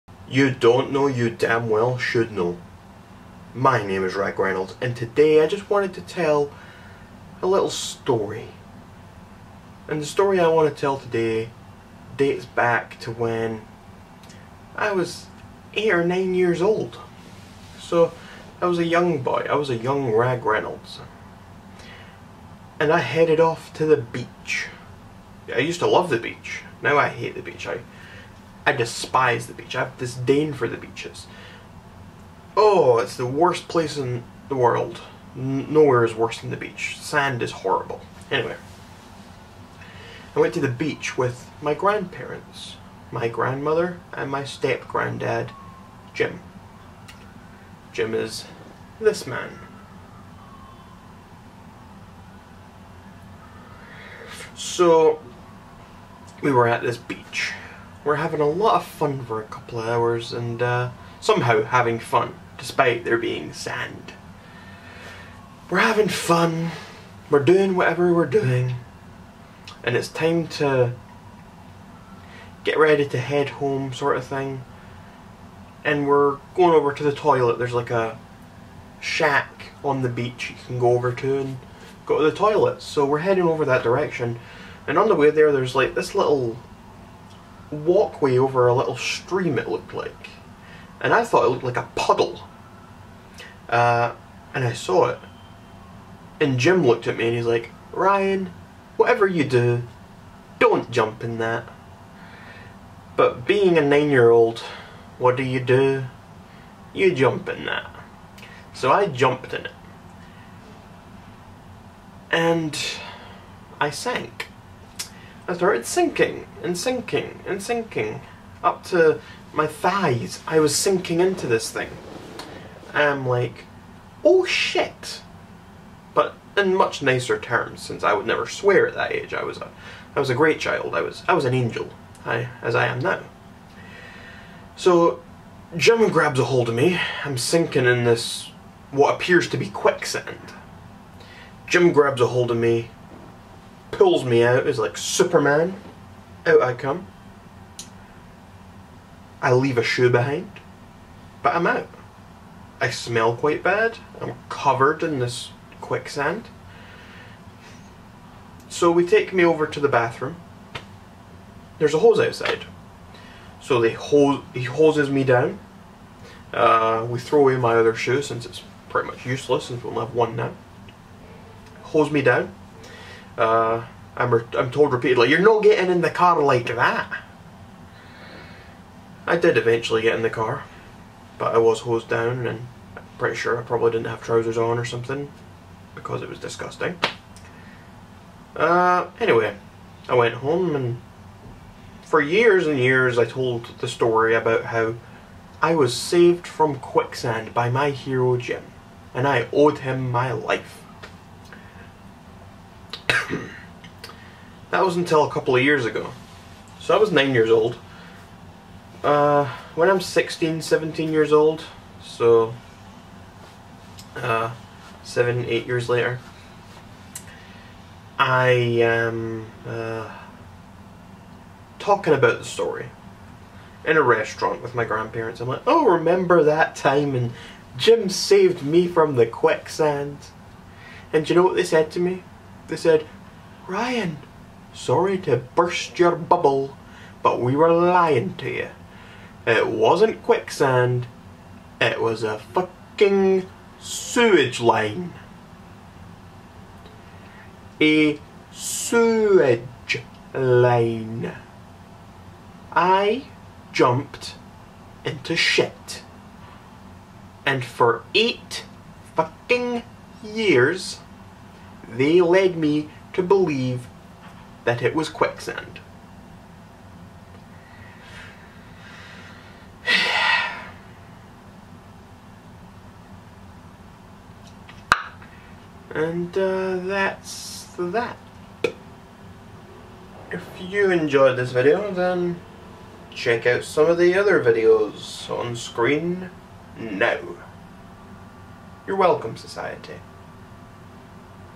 I ALMOST DROWNED IN QUICKSAND?
When I was only eight years old, I had a near death experience at the beach.